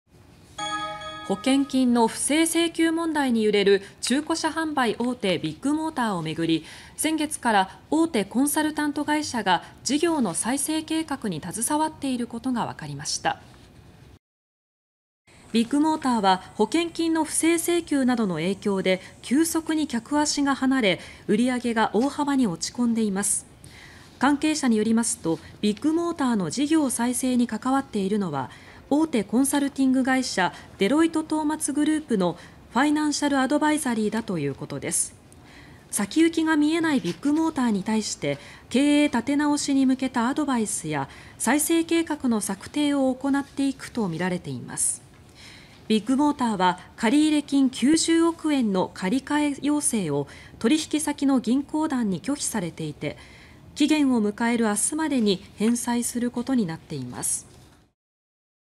【独自】大手コンサル関与で再生計画 売り上げ急減のビッグモーター(2023年8月17日)
保険金の不正請求問題に揺れる中古車販売大手「ビッグモーター」を巡り、先月から大手コンサルタント会社が事業の再生計画に携わっていることが分かりました。

　ビッグモーターは保険金の不正請求などの影響で急速に客足が離れ、売り上げが大幅に落ち込んでいます。

　関係者によりますと、ビッグモーターの事業再生に関わっているのは、大手コンサルティング会社デロイトトーマツグループの「ファイナンシャルアドバイザリー」だということです。

　先行きが見えないビッグモーターに対して経営立て直しに向けたアドバイスや再生計画の策定を行っていくとみられています。

　ビッグモーターは借入金90億円の借り換え要請を取引先の銀行団に拒否されていて、期限を迎える18日までに返済することになっています。